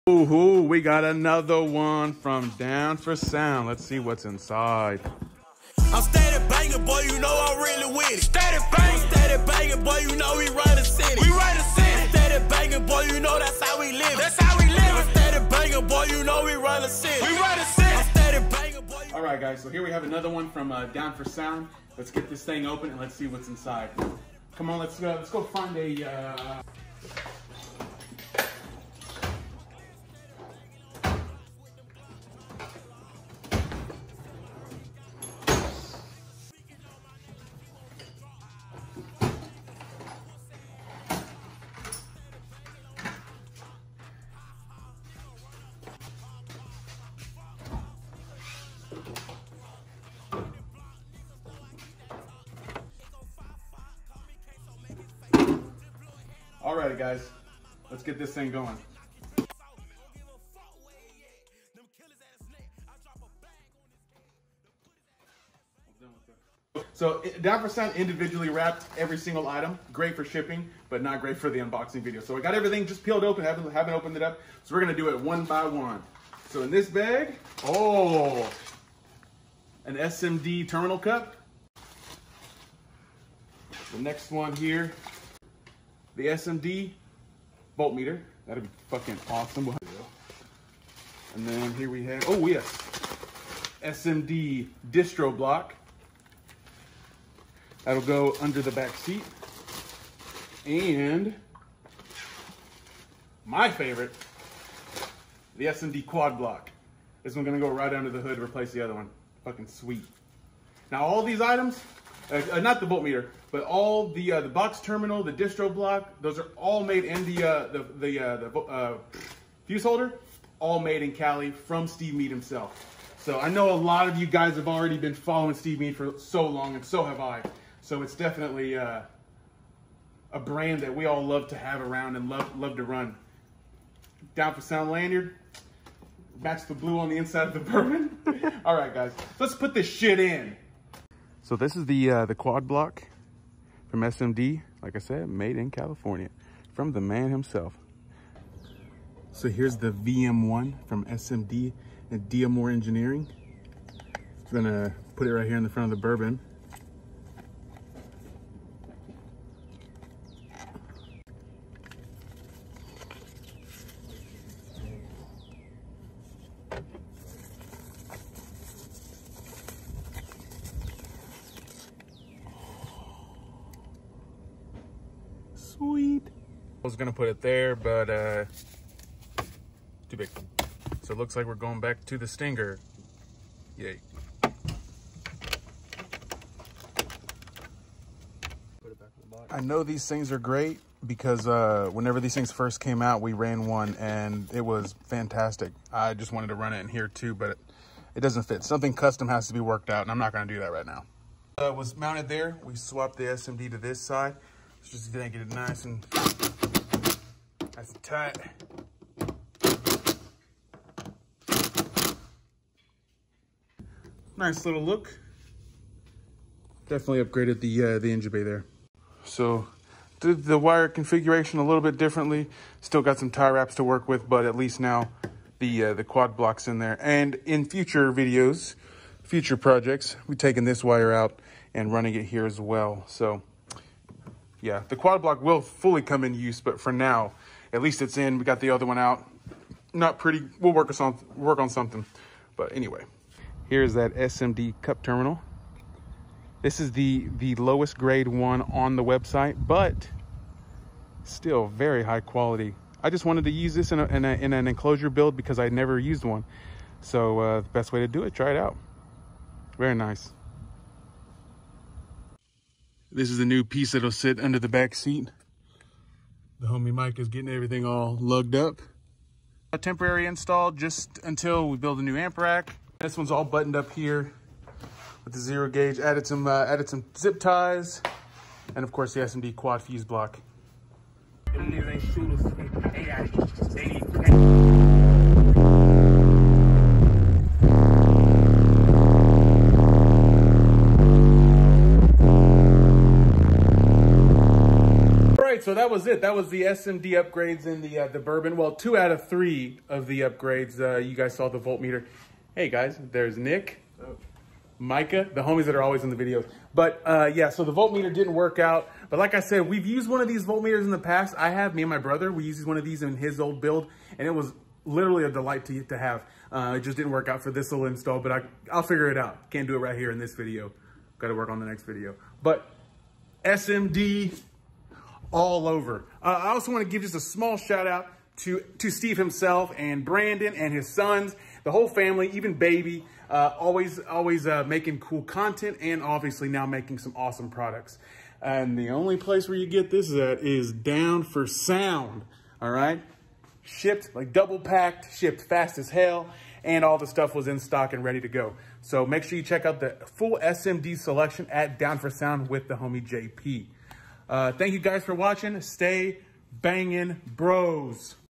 Suburban Project gets SMD! || IGTV
Here we dive down, unbox and install a few SMD items from D4S in the Suburban Project! The Quad Block fuseholder, VM-1, Half Block Distro and Terminal Cup all set for our projects here. Quality is at a top tier level, from power distribution to gain setting, @meade916 will make sure your install is supreme. Check out the entire SMD selection at down4soundshop.com @down4soundshop .

Thanks to @tst_tankthaproducer & @myka_32 for the help!!